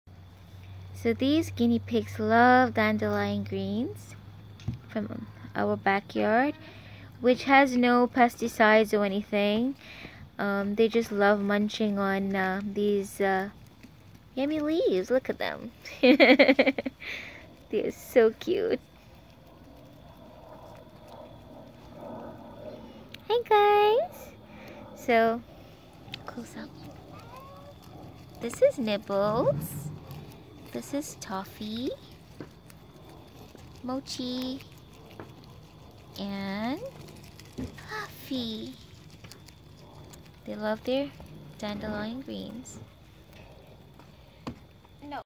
🌿DANDELION GREENS🌿🌿🌿 ARE THEIR FAV!!! NOM NOM NOM🌿🐹
🌿Dandelion leaves or greens are safe for guinea pigs to eat. Dandelion greens have a total package of different nutrients that guinea pigs need in their diet. The leaves have a high level of vitamin C which is beneficial to guinea pigs. They eat these at least 3 times a week during the Spring/Summer/Fall months while supplies last in the backyard😁🐹🌿